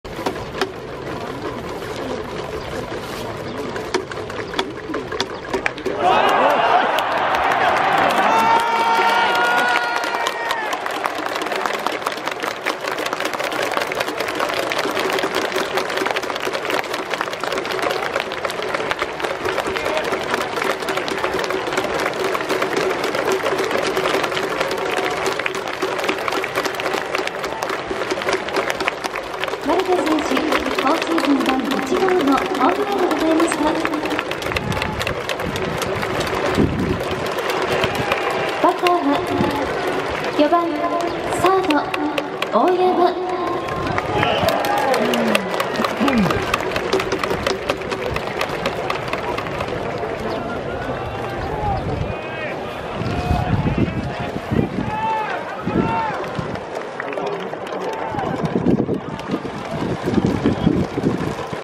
マルテ ホームラン 確信ガッツポーズ･パフォーマンスがかっこいい！
マルテのホームラン！打った瞬間、確信ガッツポーズ！パフォーマンスがかっこいいです。調子よいですね！

マルテ ホームランがものすごいスイング！バレンティンばりにスイングが捕手に当たる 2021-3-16

佐藤輝明がホームラン打った後、守備につく｡大拍手に応える姿が慣れてない様子でかわいい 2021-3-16